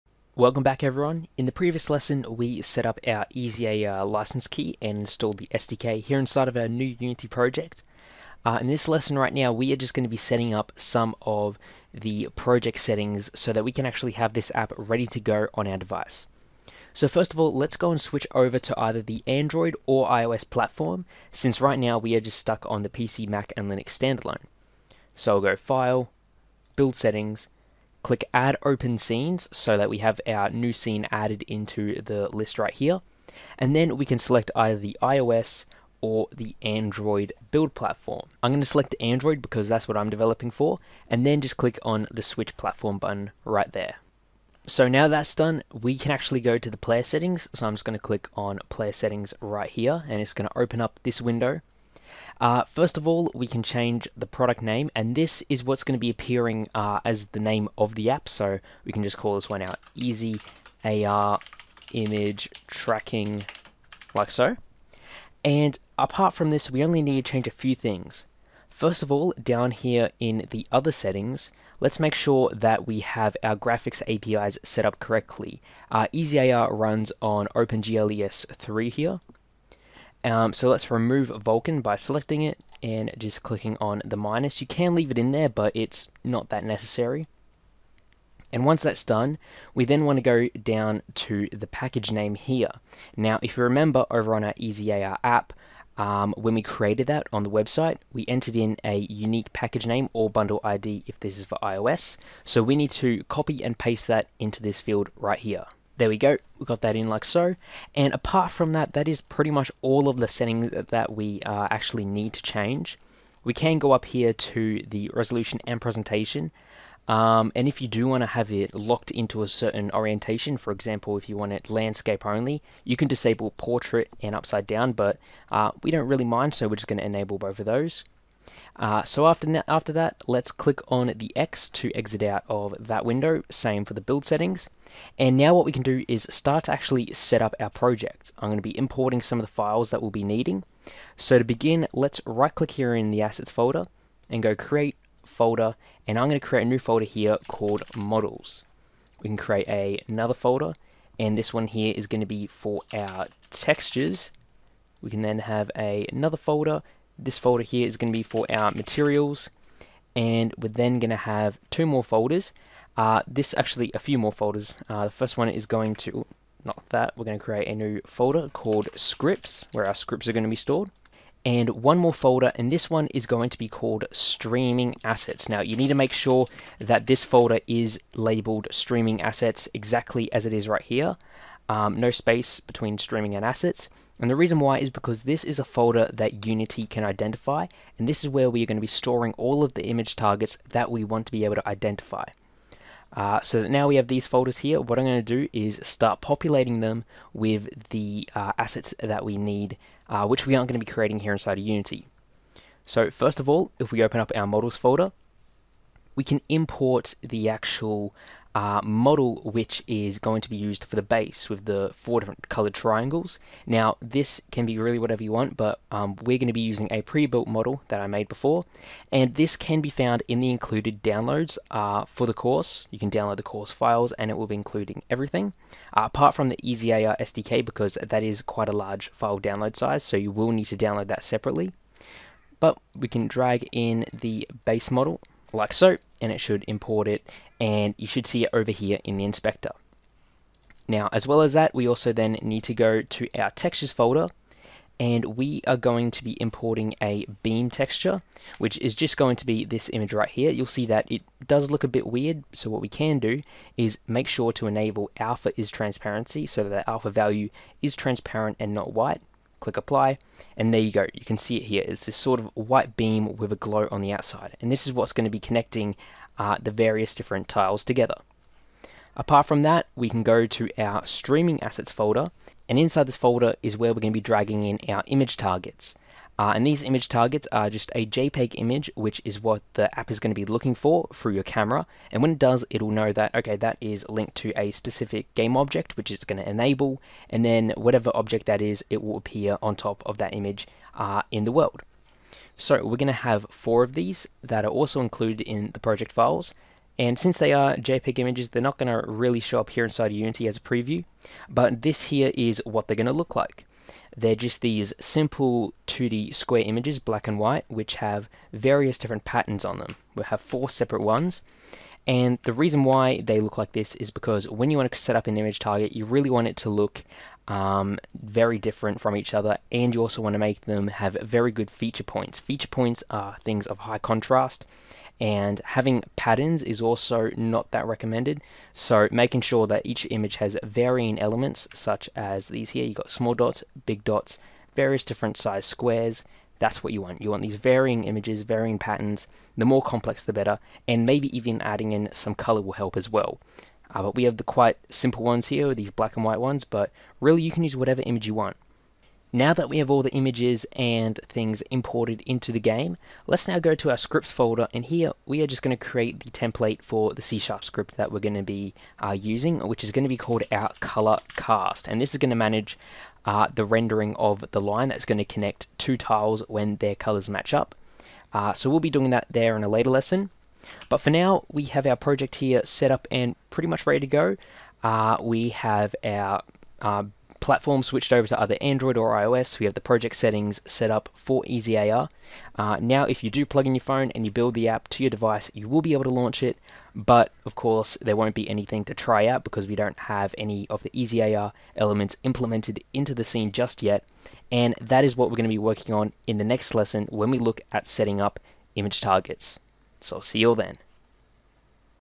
EasyAR With Unity - Project Setup
TRANSCRIPT

Welcome back everyone, in the previous lesson we set up a Easy AR license key and installed the SDK here inside of a new Unity project. And this lesson right here, we are just going to be setting up some of the project settings so that we can actually have this app ready to go on any device. So first of all, let's go and switch over to either the Android or iOS platform, since right now we are just stuck on the PC, Mac and Linux standalone. So go to File, Build Settings, click Add open scenes, so that we have our new scene added into the list right here. And then we can select either the iOS or the Android build platform. I'm going to select the Android because that's what I'm developing for. And then just click on the switch platform button right there. So now that's done, we can actually go to the Player Settings. So I'm just going to click on Player settings right here, and it's going to open up this window. First of all, we can change the product name and this is what's going to be appearing as the name of the app. So we can just call this one our EasyAR Image Tracking, like so. And apart from this, we only need to change a few things. First of all down here in the other settings, let's make sure that we have our graphics API's setup correctly, EasyAR runs on OpenGLES3 here. So let's remove Vulkam by selecting it, and just clicking on the minus, you can leave it in there but it's not that necessary. And once that's done, we then want to go down to the package name here.